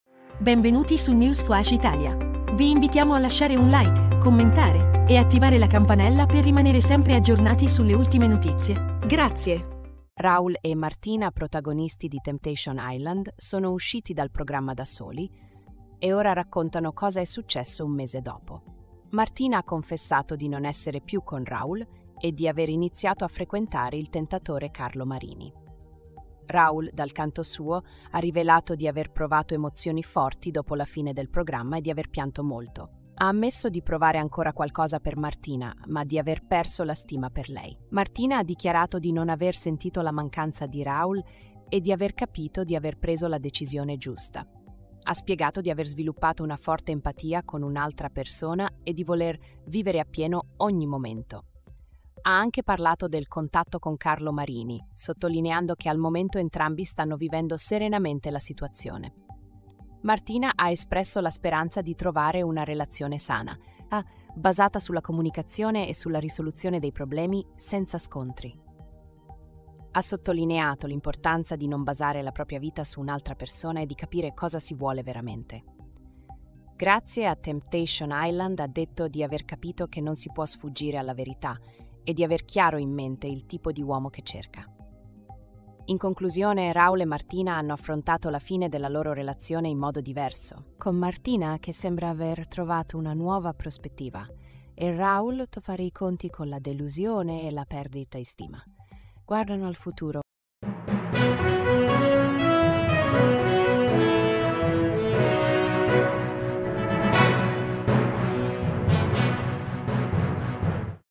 RAUL E MARTINA: UN MESE DOPO TEMPTATION ISLAND, LA VERITÀ SVELATA
Raul e Martina, protagonisti di Temptation Island, sono usciti dal programma da soli e ora raccontano cosa è successo un mese dopo. Martina ha confessato di non essere più con Raul e di aver iniziato a frequentare il tentatore Carlo Marini. Raul, dal canto suo, ha rivelato di aver provato emozioni forti dopo la fine del programma e di aver pianto molto. Ha ammesso di provare ancora qualcosa per Martina ma di aver perso la stima per lei.

Martina ha dichiarato di non aver sentito la mancanza di Raul e di aver capito di aver preso la decisione giusta. Ha spiegato di aver sviluppato una forte empatia con un'altra persona e di voler vivere appieno ogni momento. Ha anche parlato del contatto con Carlo Marini, sottolineando che al momento entrambi stanno vivendo serenamente la situazione.

Martina ha espresso la speranza di trovare una relazione sana, basata sulla comunicazione e sulla risoluzione dei problemi senza scontri. Ha sottolineato l'importanza di non basare la propria vita su un'altra persona e di capire cosa si vuole veramente. Grazie a Temptation Island, ha detto di aver capito che non si può sfuggire alla verità e di aver chiaro in mente il tipo di uomo che cerca.

In conclusione, Raul e Martina hanno affrontato la fine della loro relazione in modo diverso, con Martina che sembra aver trovato una nuova prospettiva e Raul che ha dovuto fare i conti con la delusione e la perdita di stima. Entrambi guardano al futuro con speranza e consapevolezza.